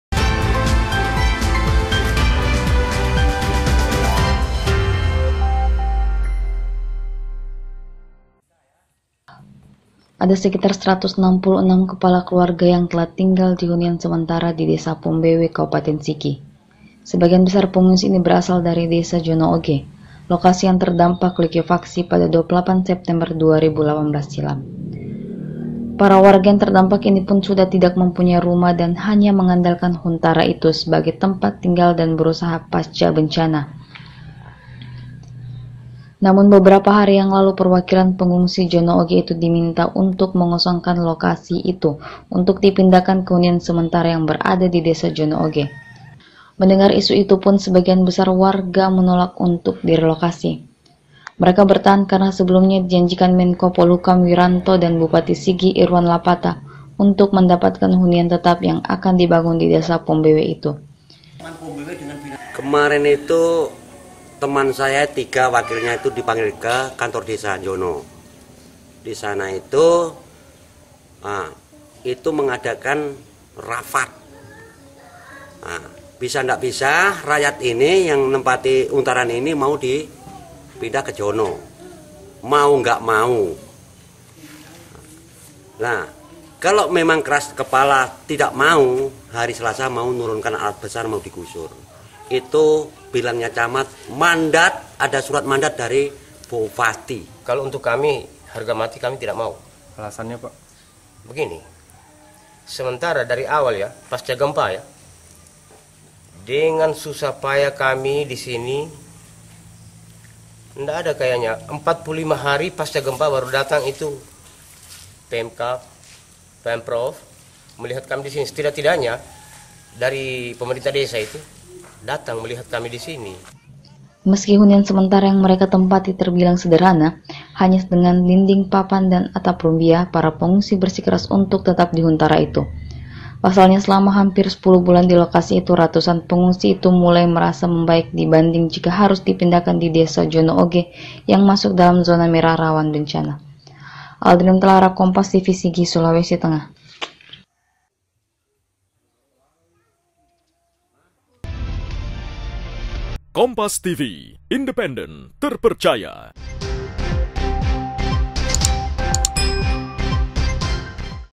WARGA JONO OGE MENGELUH AKAN DIRELOKASI LAGI, KOMPASTV PALU
RATUSAN PENGUNGSI ASAL DESA JONO OGE TERANCAM AKAN DIRELOKASI DARI HUNIAN SEMENTARA DI DESA POMBEWE KABUPATEN SIGI SULAWSI TENGAH. PADAHAL SUDAH HAMPIR SEPULUH BULAN KORBAN LIKUEFAKSI JONO OGE ITU MENEMPATI HUNIAN SEMENTARA YANG DIBANGUN SECARA GOTONG ROYONG OLEH WARGA SETEMPAT.